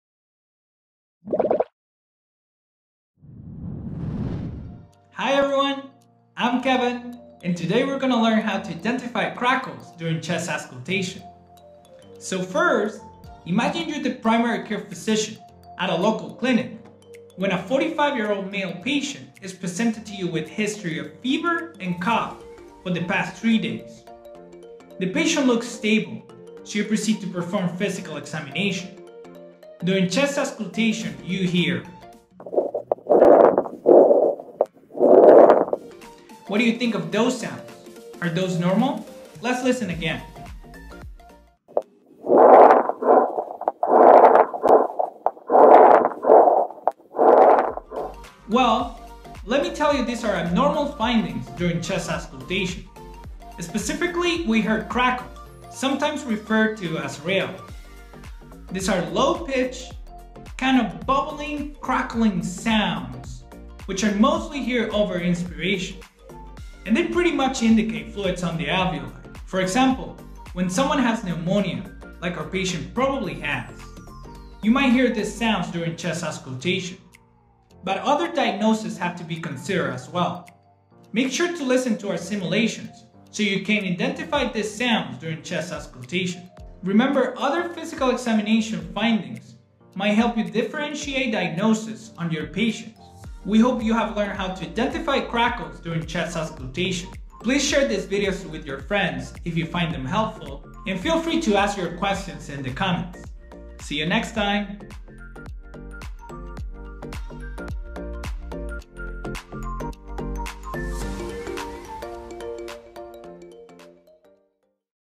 Identify crackels during chest auscultation, by Body Interact Virtual Patients
Clinical Tips - Reviewing Medical Concepts
In this third episode, we will learn how to identify wheezing during chest auscultation

Clinical Tips is a series of videos created especially for medical students who are looking to review concepts in this area of clinical practice.

Also, take the chance to experience Body Interact for free with our free